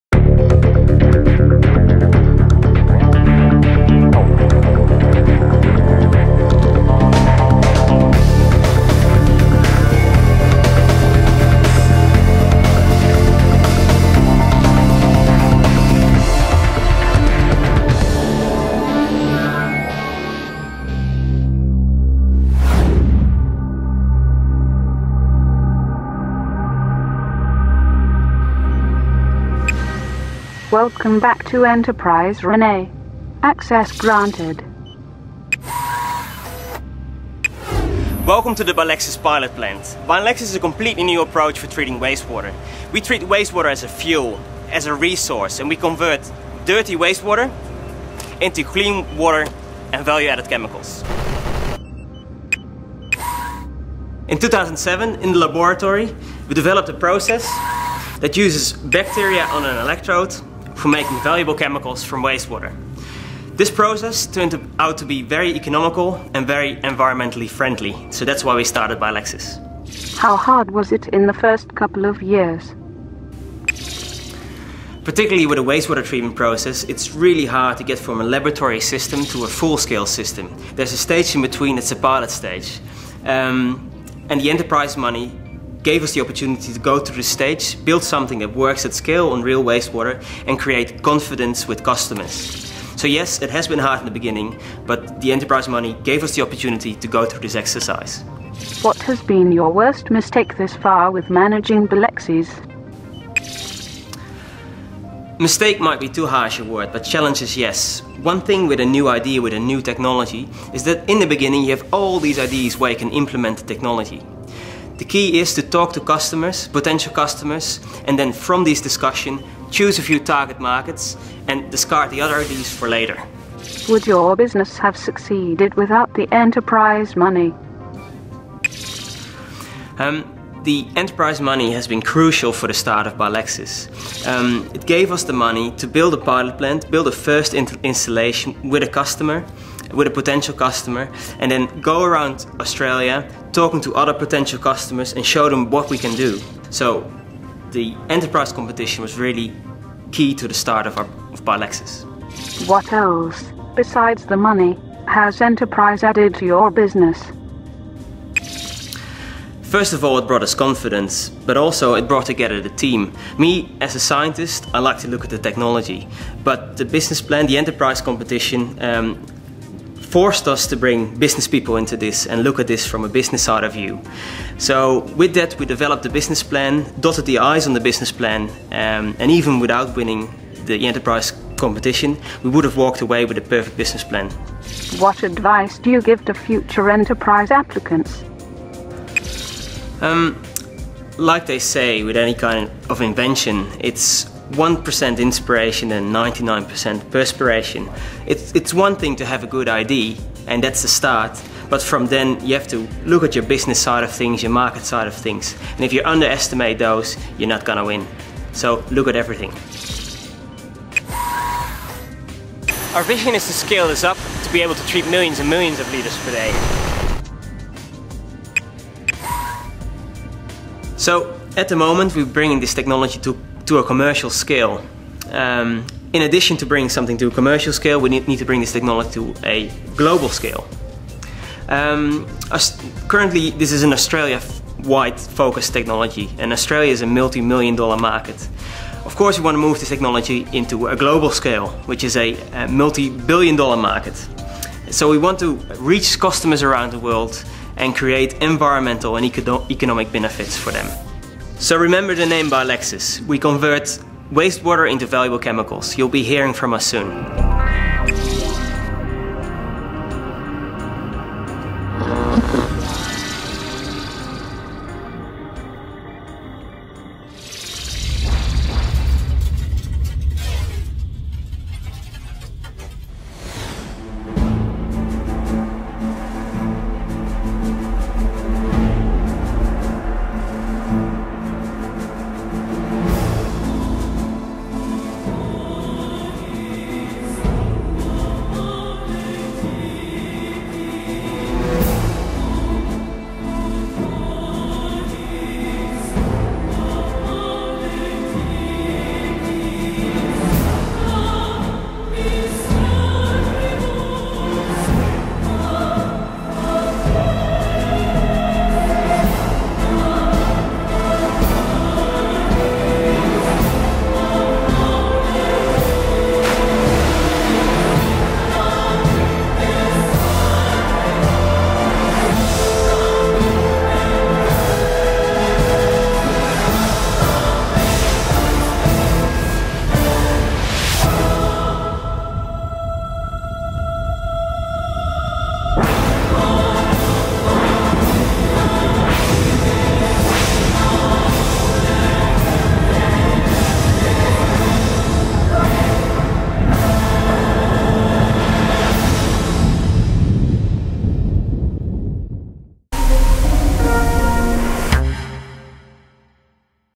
2008 WINNER: BILEXYS.
In 2007 in the laboratory we developed a process that uses bacteria on an electrode for making valuable chemicals from waste water.